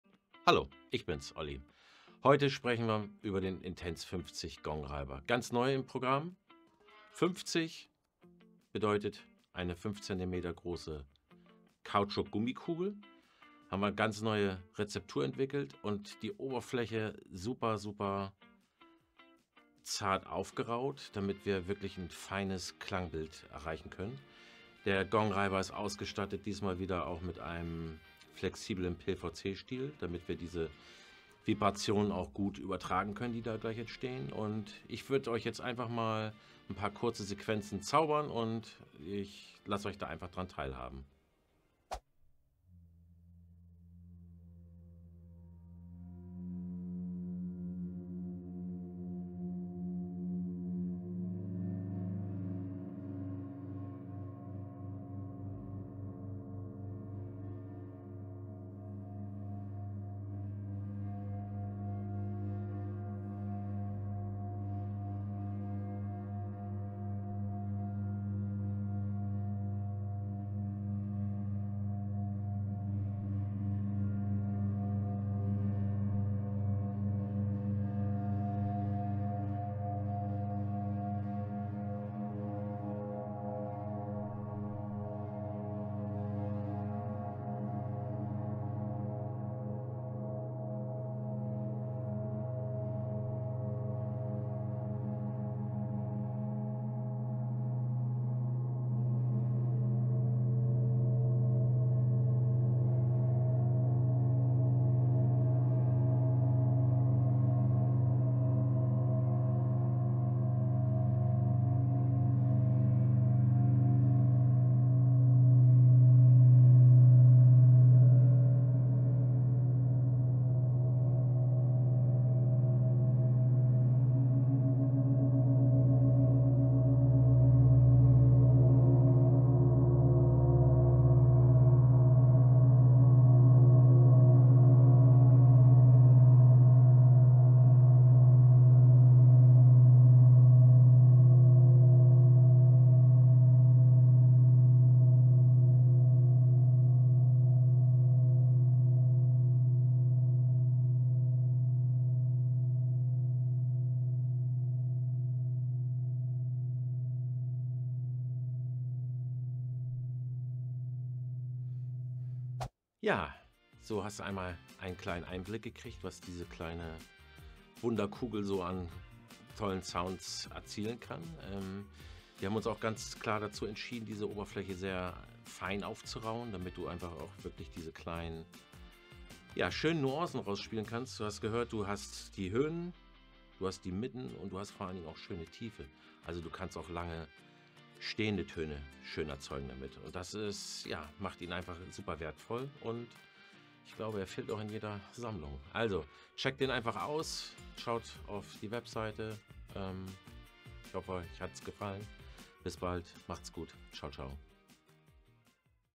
Intense Edition 50: Der Gongreiber für sofortige kraftvolle Klänge | ollihess
Mit der ollihess Intense Edition erlebst du eine neue Dimension der Klangvielfalt! Dieser Gongreiber bietet dir sofort intensive Klänge und ist ideal, um mittlere bis tiefe Töne zu erzeugen. Überzeug dich selbst von der präzisen Handarbeit und den vielseitigen Einsatzmöglichkeiten!

▶▶ Welche Klänge sind mit dem ollihess Intense 50 Gongreiber möglich?

Das Besondere an der Intense Edition ist die extrem fein aufgeraute Oberfläche. Diese ermöglicht dir eine enorme Klangvielfalt. Mit diesem Gongreiber erzeugst du sofort intensive tiefe und warme Töne und kannst sogar feinste Nuancen aus deinem Gong herausholen. Schöpfe das volle Soundspektrum deines Gongs aus: Spiele auf dem Rand oder in der Mitte deines Gongs – die Intense Edition liefert dir kraftvolle, langanhaltende Sounds, die dein Spiel auf ein neues Level heben.

▶▶ Für wen ist die ollihess Intense Edition 50 geeignet?

Die Intense Edition 50 ist perfekt für Musiker:innen, Klangtherapeut:innen oder Meditationslehrer:innen – sowie alle, die tiefe und mittlere Klänge in ihre Gongpraxis integrieren möchten. Du bekommst die Intense Edition in unterschiedlichen Kopfgrößen – dadurch hast du stets die volle Kontrolle über dein Klangspektrum.

▶▶ Top-Features des ollihess Intense Edition 50 Gongreibers

💡 Fein aufgeraute Oberfläche für sofortige intensive Klänge
💡 Präzise Spieltechnik dank komfortablem Handling
💡 Ideal für mittlere bis tiefe Töne und feine Klangnuancen
💡 Nachhaltig und fair in Deutschland produziert

▶▶ Ollis Ausrüstung in diesem Video

▶▶ Du willst noch mehr über hochwertige Mallets & Gongreiber, besondere Spieltechniken und die besten Materialien erfahren?